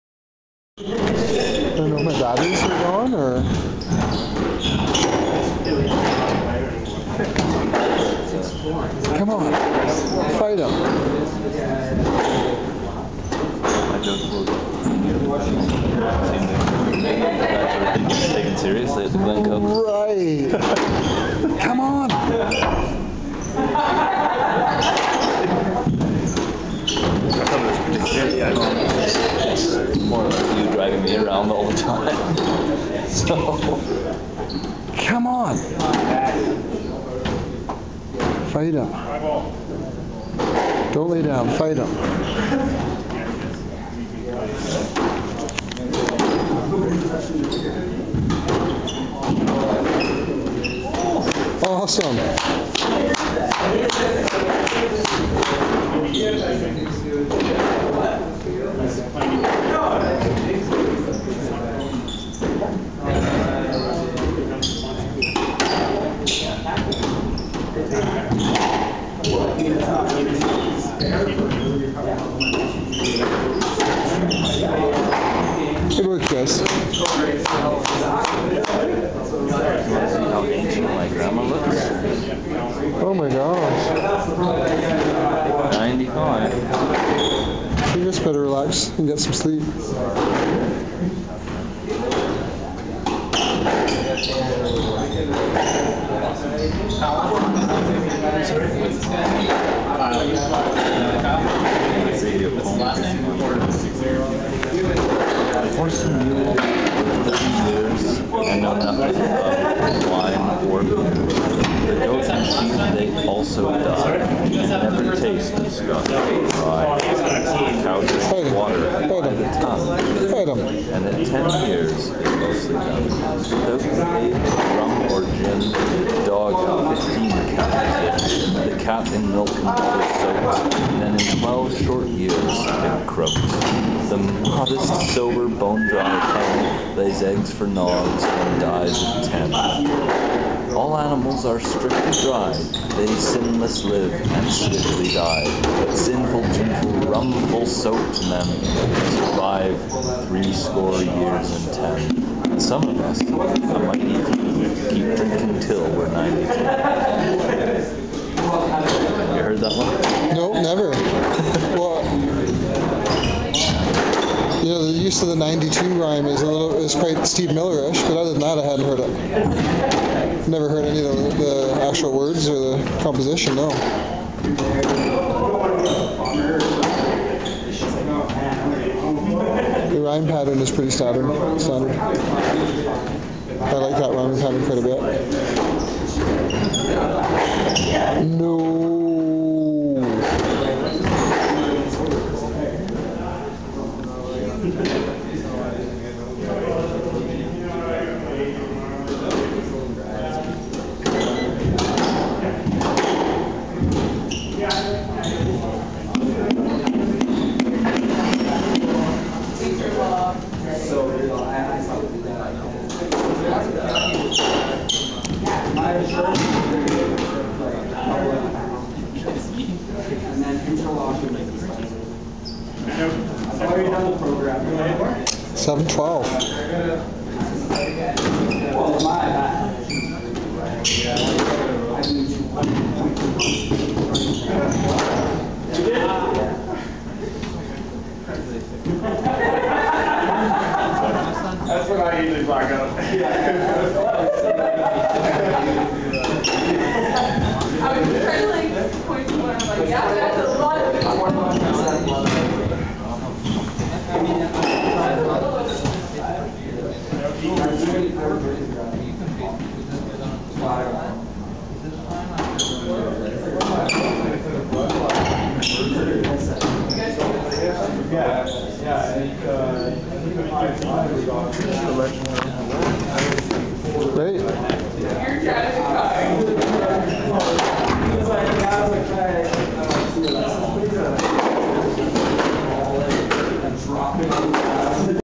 K. Porter v. B.
Calgary Interclub Squash League: Men's Level 1 - University of Calgary v. Glencoe